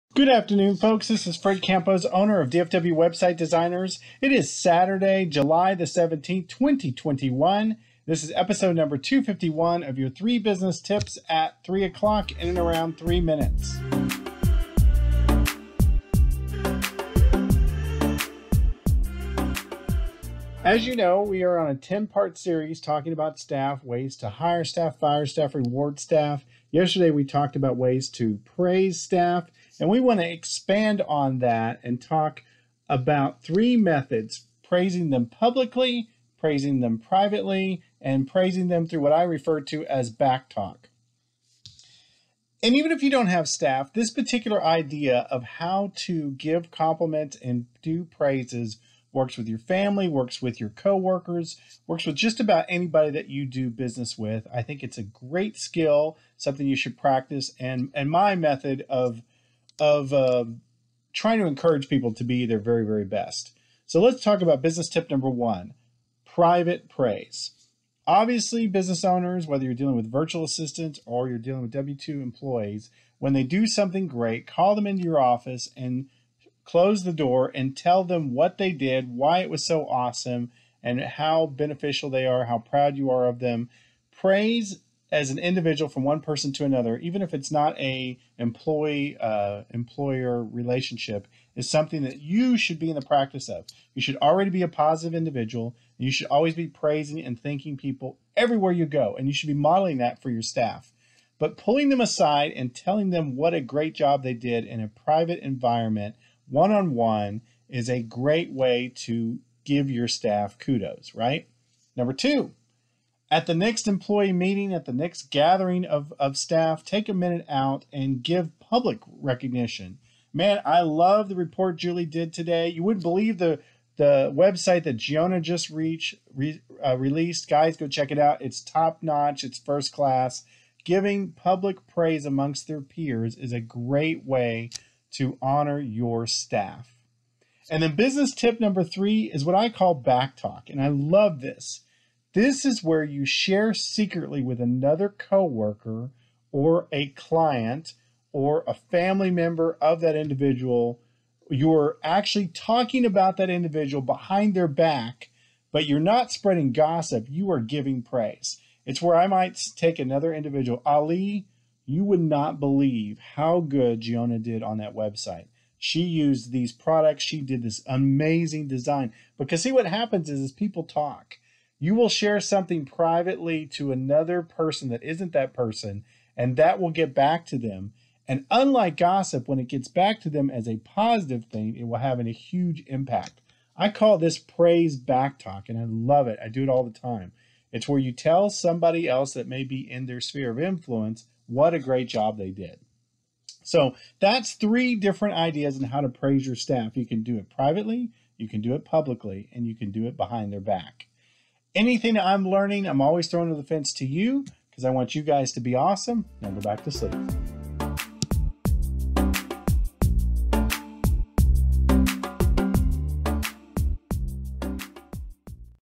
3:3:3 Episode 251 Staff Praise - Private, Public, Back Talk
Episode 251 of my 3 Business​​​​​​​ Tips​​​​​​​ in 3 Mins at 3p CST. Staff Praise - Private Praise, Public Praise, Praise Back Talk 07/17/2021